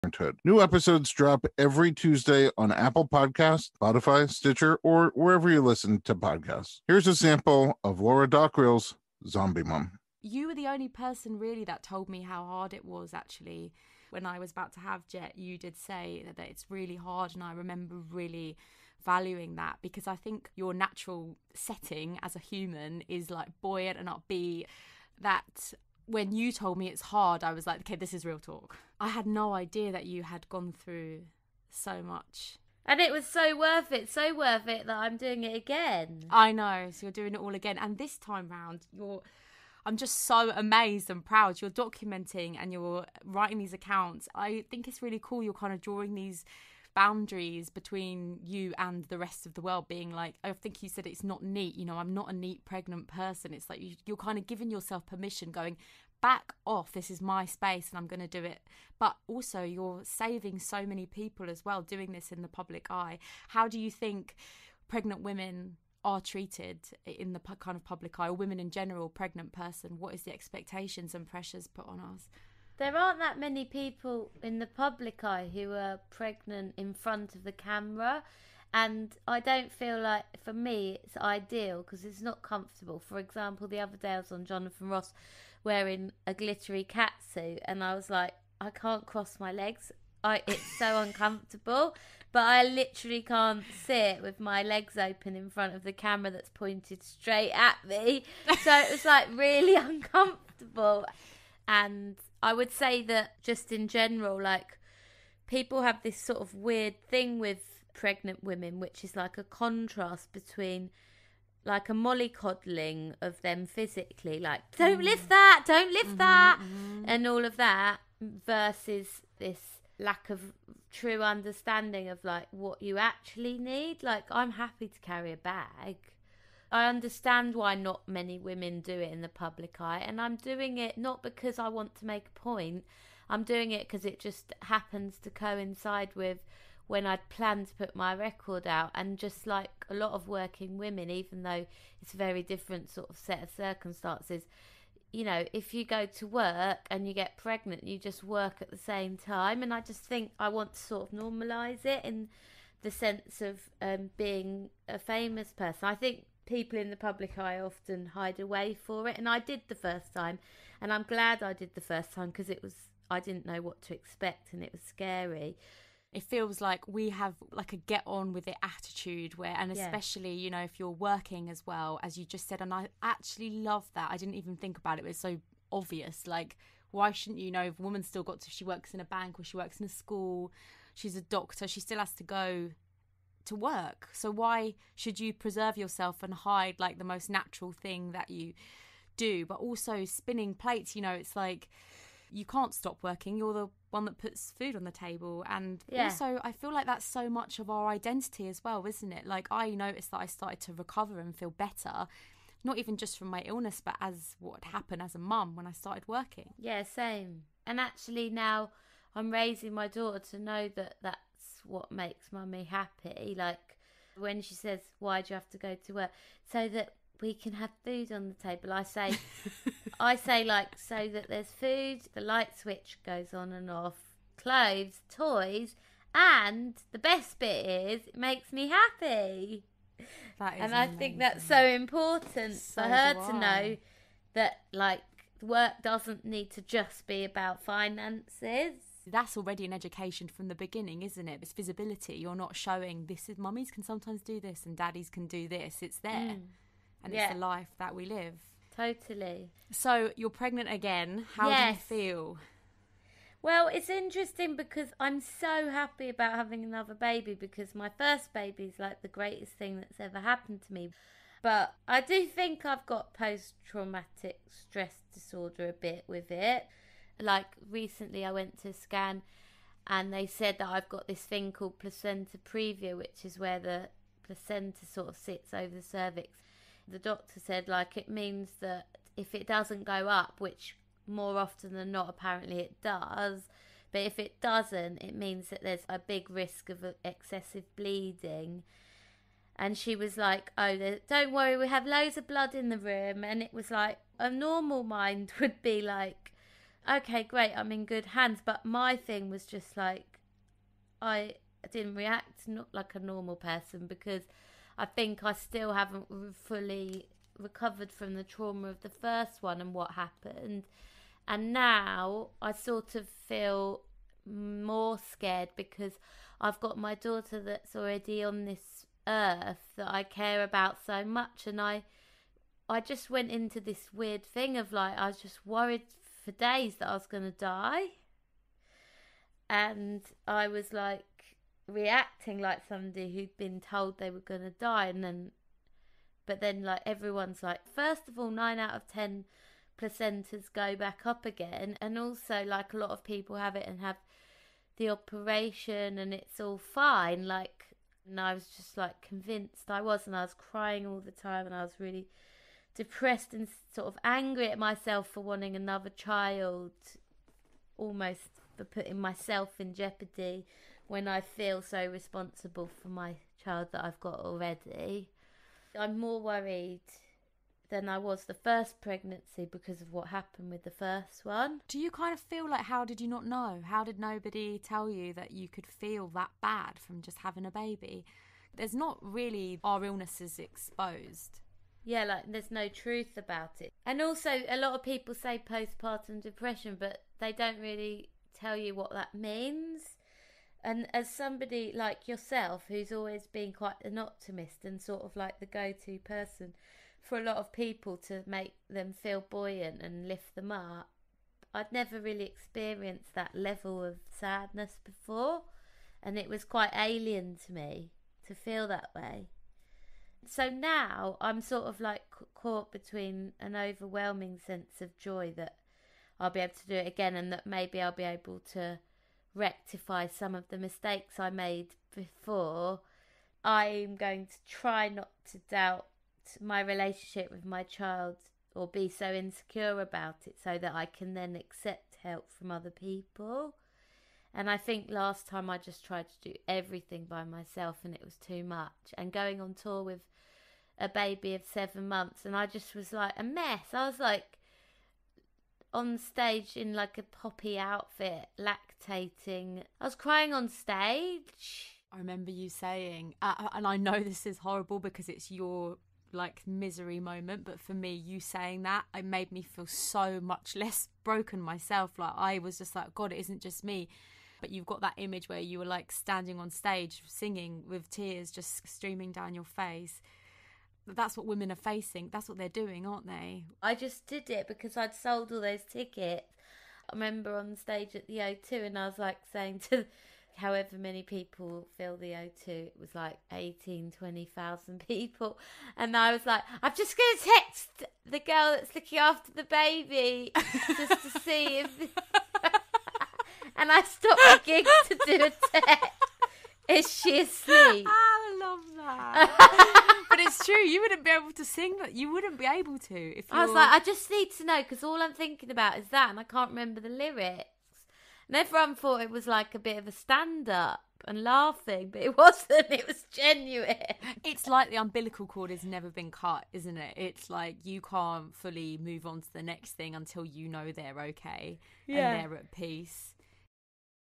Ep. 222 Zombiemum┃Informed Pregnancy Podcast
Ep. 222 Zombiemum

After her startling experience with postpartum psychosis, writer Laura Dockrill realized there were plenty of aspects of motherhood that need more discussion and awareness.  On her new weekly podcast, Zombiemum, Laura is joined by fellow parents who also didn’t have the picture-perfect journey they anticipated. Whatever you’re feeling, you’re not alone.

Connect with Informed Pregnancy Media